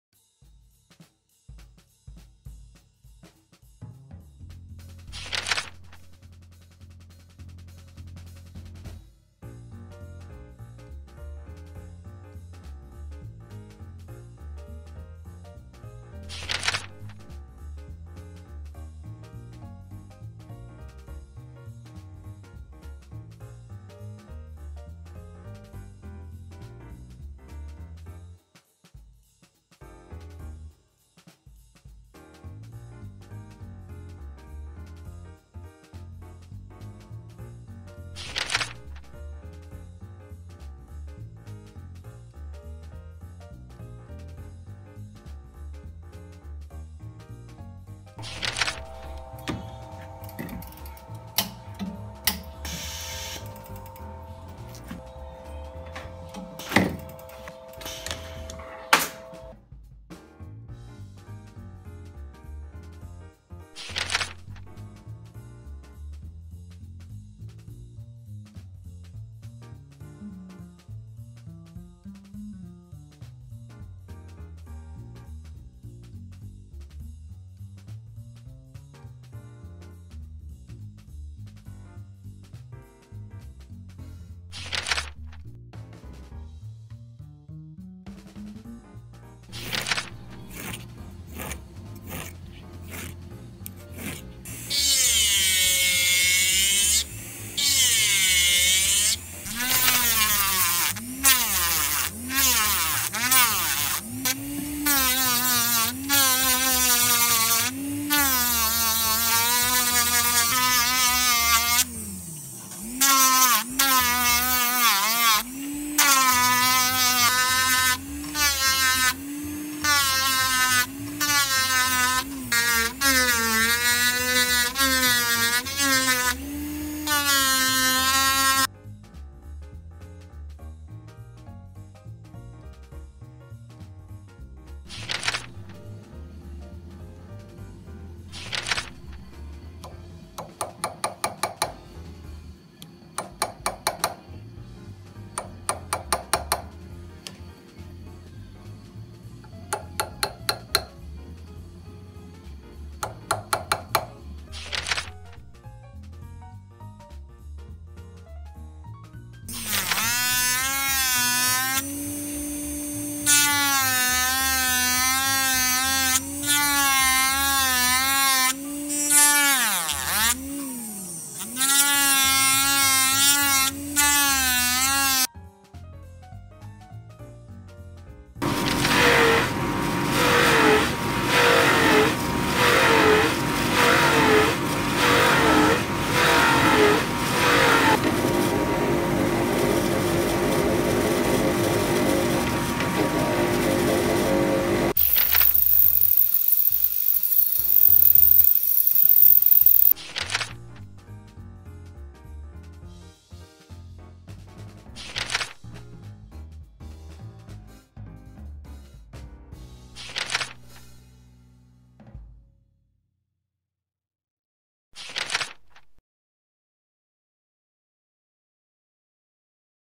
【ジュエリーリフォーム】立て爪が邪魔なダイヤモンドプラチナリングを覆輪リングに作り替える
お客様お持ち込みの大きなダイヤモンドが付いたプラチナリングをジュエリーリフォームしました。
婚約指輪として頂いたものだそうですが大きな立て爪が邪魔で着用出来ないので衣類等に引っ掛からないように作り替えて欲しいということで作業させて頂きました。

この覆輪リングはシルバー925バージョンで商品化しておりますので気になる方は詳細は下記URLにて